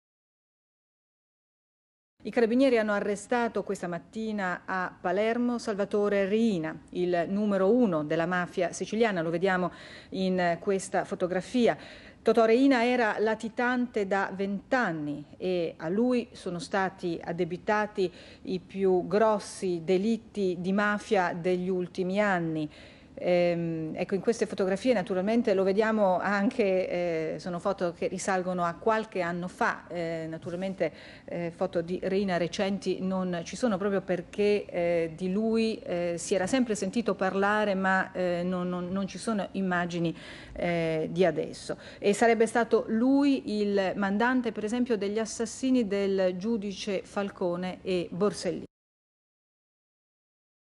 LA NOTIZIA DELLA CATTURA DI TOTO' RIINA - 15 GENNAIO 1993 (EDIZIONE STRAORDINARIA)
La mattina del 15 gennaio 1993, il boss di "Cosa Nostra", Salvatore (detto Totò) Riina, venne catturato in via Regione Siciliana, a Palermo, assieme al suo autista, Salvatore Biondino.
L'operazione venne portata a termine dai carabinieri del Crimor, una squadra speciale dei ROS, coadiuvati dal "Capitano Ultimo" avvalendosi anche della collaborazione di alcuni pentiti, in particolar modo Baldassarre (detto "Balduccio") Di Maggio, uno tra i pochi in grado di riconoscerlo.
Totò Riina, ritenuto l'artefice delle peggiori stragi verificatesi in Sicilia tra gli anni '70, '80 e '90 (tra cui quella di Capaci e di via D'Amelio dove morirono rispettivamente i giudici Giovanni Falcone e Paolo Borsellino) era ormai latitante da quasi 25 anni